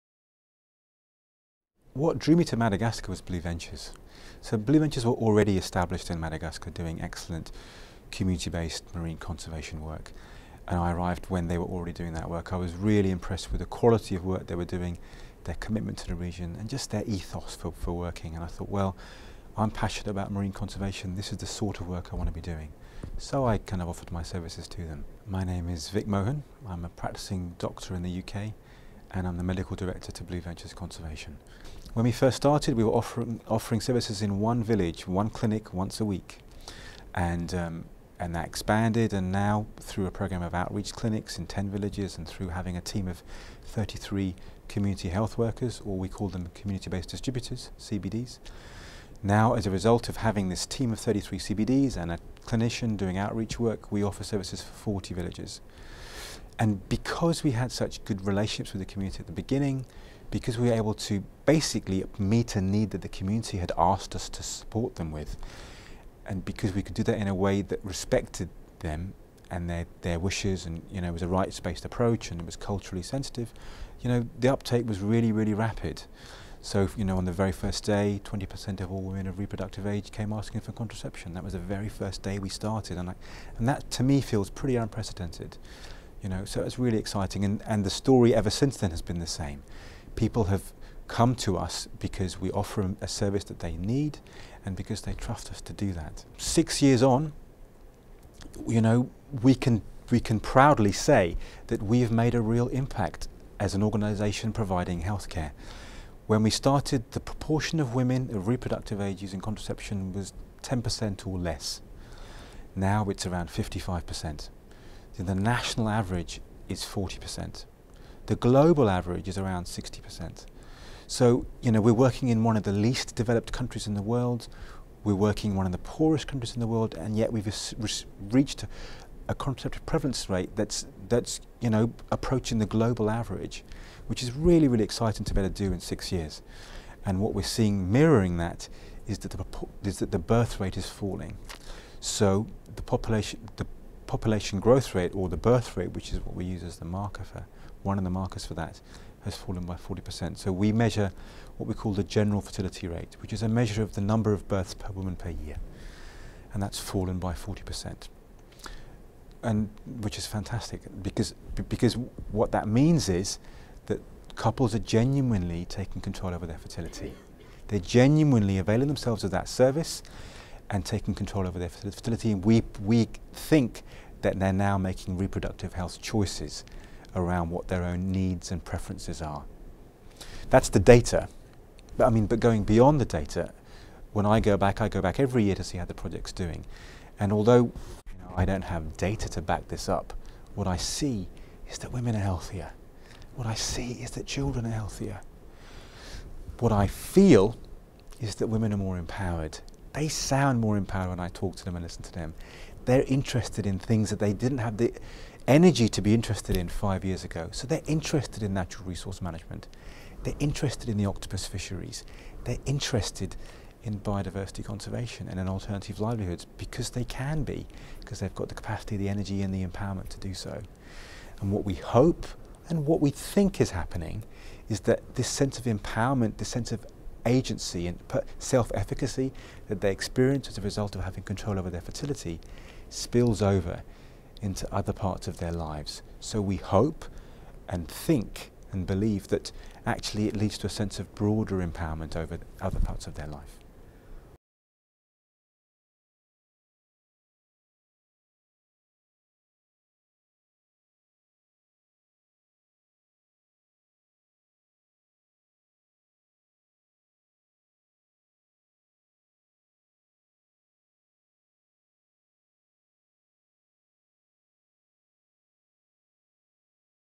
Vik Mohan: "What I See Is That Women Are Healthier...Children Are Healthier"
Six years after beginning a marine conservation program focused on octopus fishery management in southwest Madagascar, "we can proudly say that we have made a real impact as an organization providing health care," said Dr. Vik Mohan, medical director of Blue Ventures and a practicing doctor in the United Kingdom.
"What drew me to Madagascar was Blue Ventures," said Mohan, "I was really impressed with the quality of work they were doing, their commitment to the region, and just their ethos for working." After working for some time solely on marine conservation in a number of villages in Velondriake, the eco-tourism NGO saw rapid population growth putting pressure on natural resources, leading to malnutrition and maternal health problems. In response, the group adopted a more comprehensive approach to conservation and added a clinic to meet demand for reproductive health services and empower women.